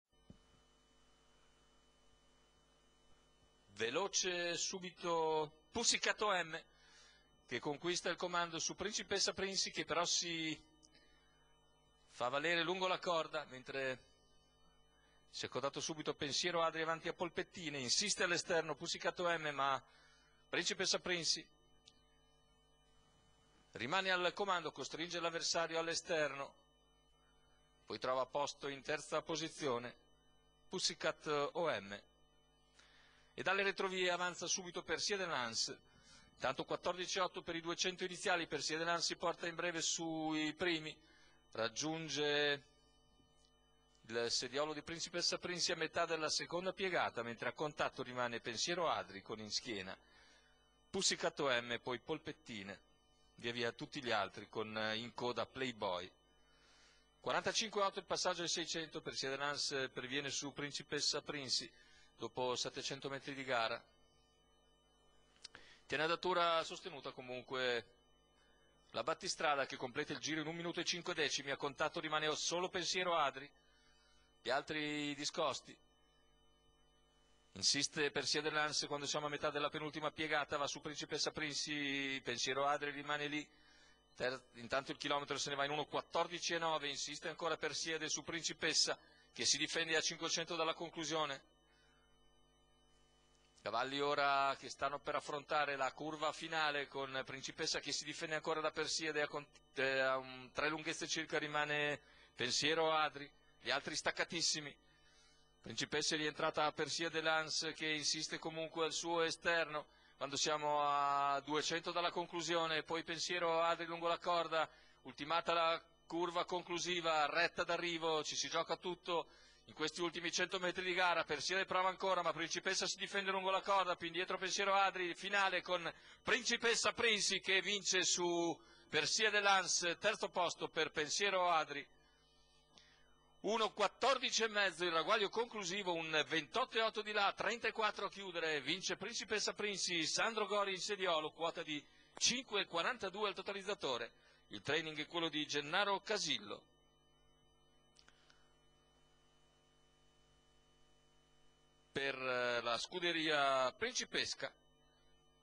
08/12/2013, Bologna , Premio Club Gentlemen Em.romagna
1. PRINCIPESSAPRINCY (GORI SANDRO), 2. PERSIADE LANS (ALESSI L.), 3. PENSIERO ADRI (DE LUCA M.), 4. POLPETTINE (MONTI F.), 5. PUSSYCAT OM (BELLUCCI P.), 6. PACO (GHIRARDINI G.), 7. PANAMA TRIO (BARBIERI WERTHER), 8. PLAY BOY (ROSSI MICH.), 9. PARA NO VERTE MAS (MELI C.)